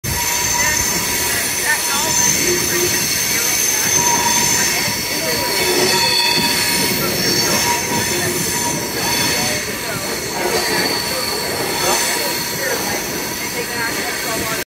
Tom Ryan Royal Gorge Rafting the Arkansas River
River rafting on the Arkansas River, video taken from aboard the Royal Gorge Railroad